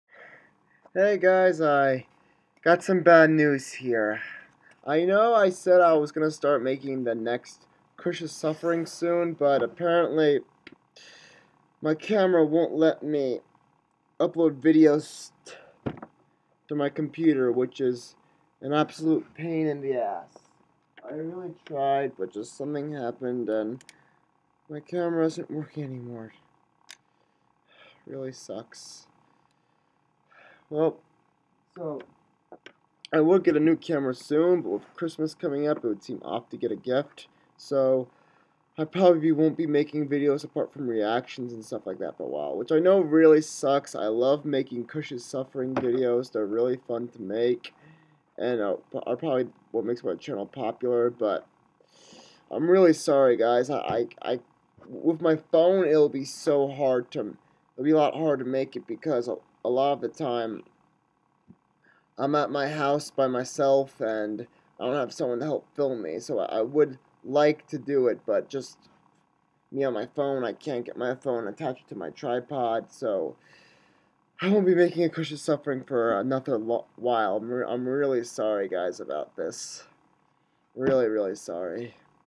Major Update CAMERA BROKEN!!!
My camera just suddenly stopped uploading to computers for some reason and I can't make Cush's Suffering for a while.